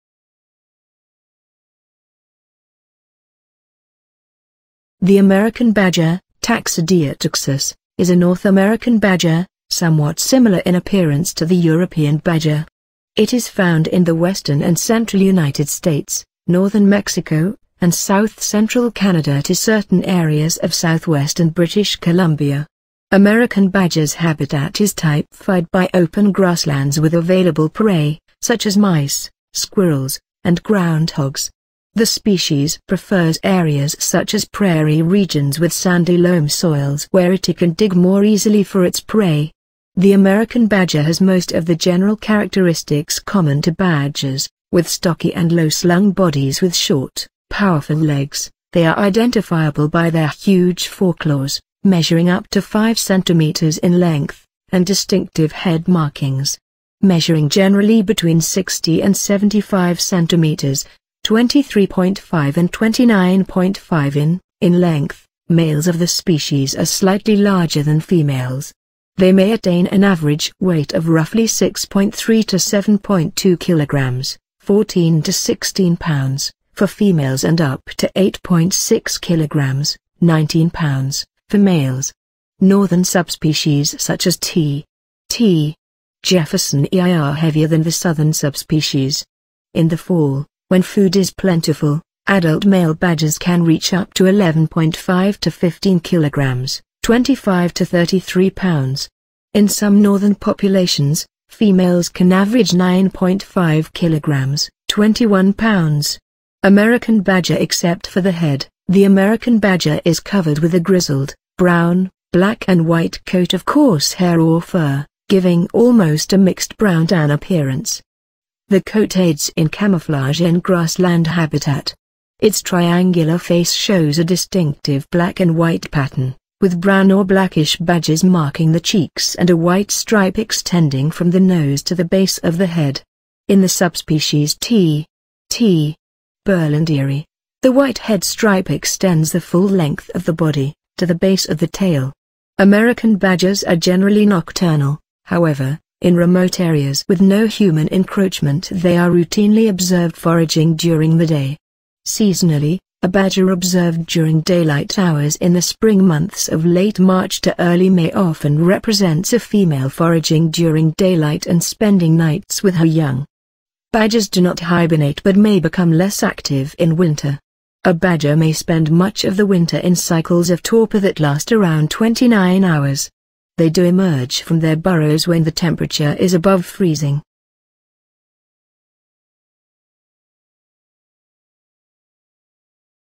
Interesting facts about BADGER by Animals and Insect TV
Just be happy and Enjoy...

The American badger (Taxidea taxus) is a North American badger, somewhat similar in appearance to the European badger. It is found in the western and central United States, northern Mexico, and south-central Canada to certain areas of southwestern British Columbia.
American badger's habitat is typefied by open grasslands with available prey (such as mice, squirrels, and groundhogs). The species prefers areas such as prairie regions with sandy loam soils where it can dig more easily for its prey.
The American badger has most of the general characteristics common to badgers; with stocky and low-slung bodies with short, powerful legs, they are identifiable by their huge foreclaws (measuring up to 5 cm in length) and distinctive head markings. Measuring generally between 60 and 75 cm (23.5 and 29.5 in) in length, males of the species are slightly larger than females. They may attain an average weight of roughly 6.3 to 7.2 kg (14 to 16 lb) for females and up to 8.6 kg (19 lb) for males. Northern subspecies such as T. t. jeffersonii are heavier than the southern subspecies. In the fall, when food is plentiful, adult male badgers can reach up to 11.5 to 15 kg (25 to 33 lb). In some northern populations, females can average 9.5 kg (21 lb).
American badger
Except for the head, the American badger is covered with a grizzled, brown, black and white coat of coarse hair or fur, giving almost a mixed brown-tan appearance. The coat aids in camouflage in grassland habitat. Its triangular face shows a distinctive black and white pattern, with brown or blackish "badges" marking the cheeks and a white stripe extending from the nose to the base of the head. In the subspecies T. t. berlandieri, the white head stripe extends the full length of the body, to the base of the tail.
American badgers are generally nocturnal; however, in remote areas with no human encroachment they are routinely observed foraging during the day. Seasonally, a badger observed during daylight hours in the Spring months of late March to early May often represents a female foraging during daylight and spending nights with her young. Badgers do not hibernate but may become less active in winter. A badger may spend much of the winter in cycles of torpor that last around 29 hours. They do emerge from their burrows when the temperature is above freezing. Animal planet | Discovery | Netflix | Documentary | Animal World
Animals and Insect TV
License